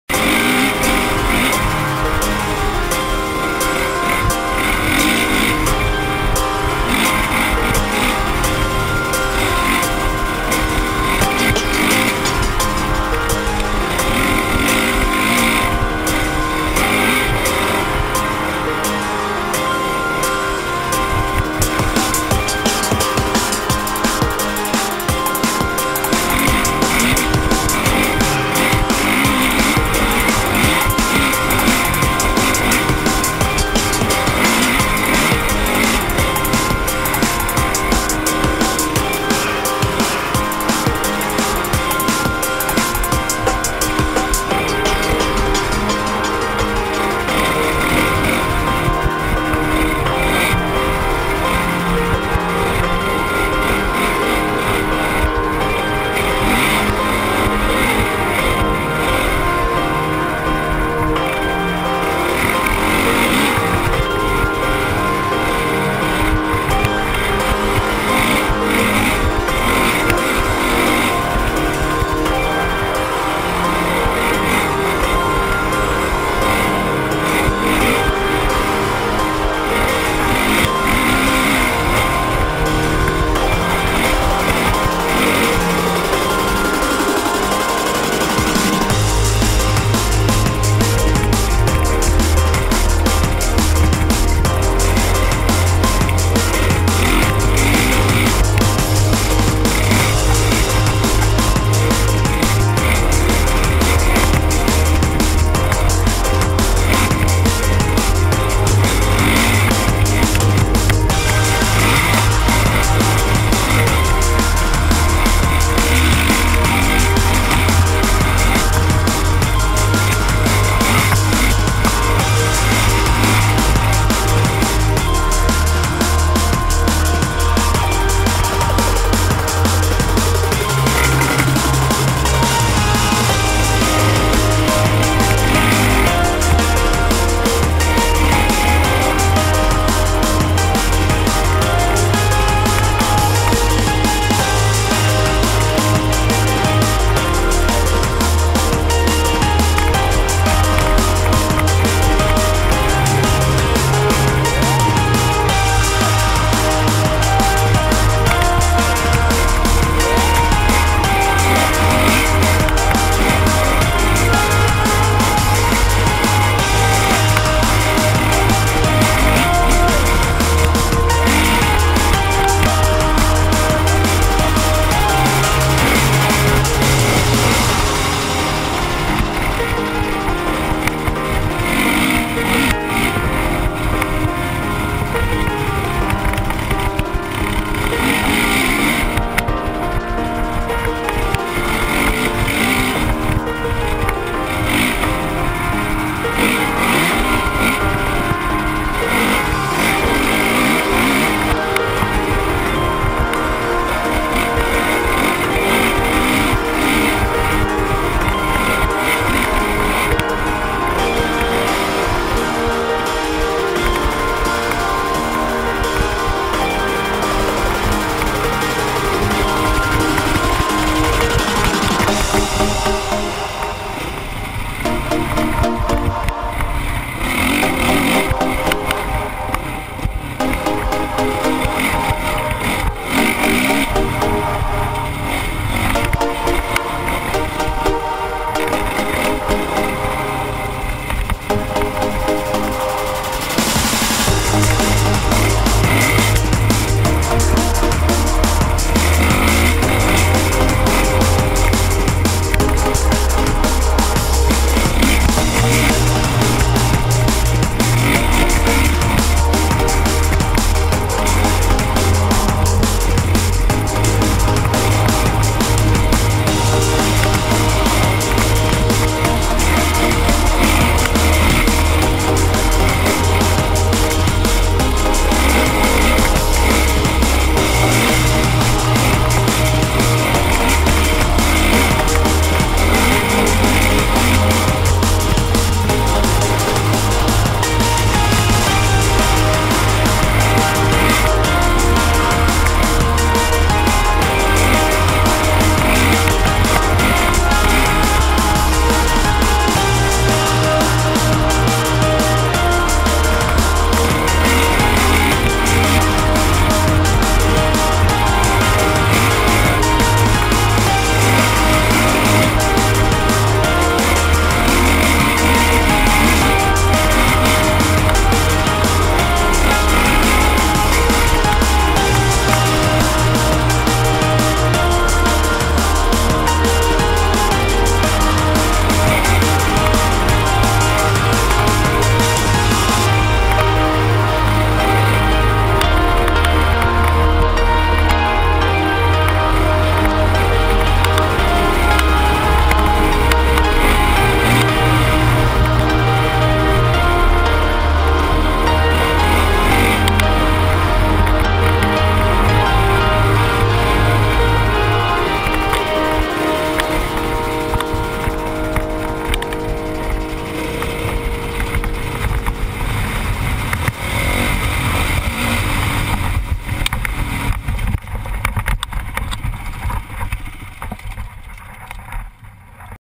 Gopro HD: Dirt Bike Ridge Riding New Zealand
This track was on Glen Nui trail ride in Taranaki, New Zealand.
Loose straps on my bag made a lot of annoying noise so i put some music in...and the footage was shaky as hell too so i stabilised it in the editor which helped...anyway enough of my rambling haha hope you enjoy the vid!

Bike: 08 CRF450R
Camera: Gopro HD